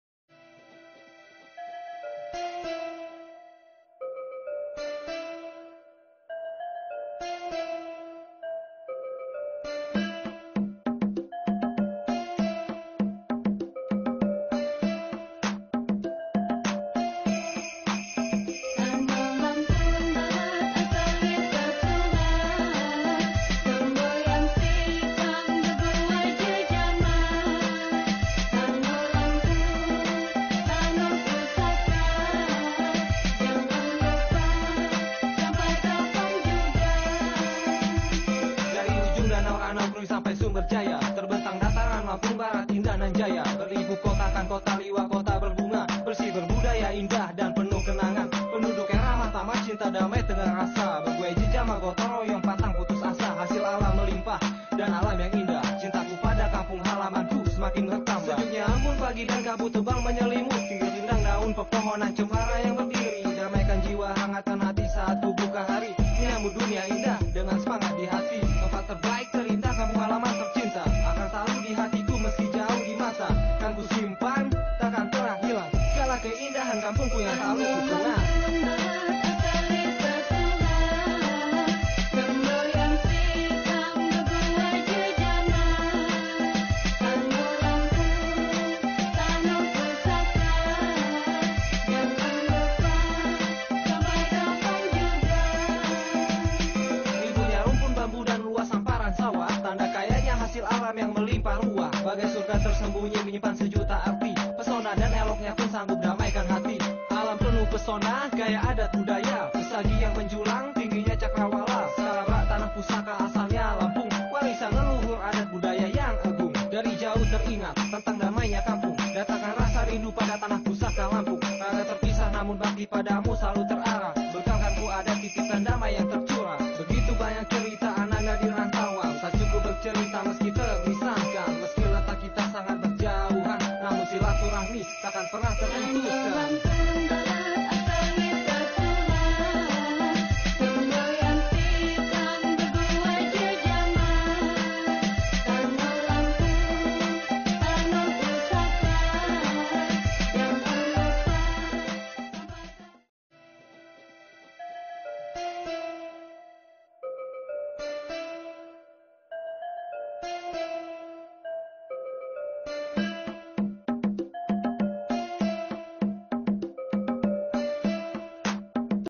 LA MÁSCARA DE MIL FIESTA AVANZADA DEL PUEBLO EN EL ESTADO DE INDONESIA, PROVINCIA DE LAMPUNG
LA MÁSCARA DE MIL FUNDACIÓN DE UN PARTIDO PUEBLO EN EL ESTADO DE INDONESIA, LA PROVINCIA DE LAMPUNG QUE USUALMENTE TIENE POBLACIONES LOCALES DESPUÉS DE MILES DE CELEBRACIONES HUMANAS

pesta sekura cakak buah atau panjat  pinang adalah salah satu tradisni unik tahunan yang di lakasanakan setahun sekali yaitu di hari raya idul fitri atau lebaran setelah selesai mengikuti puasa satu bulan penuh di bulan ramadan.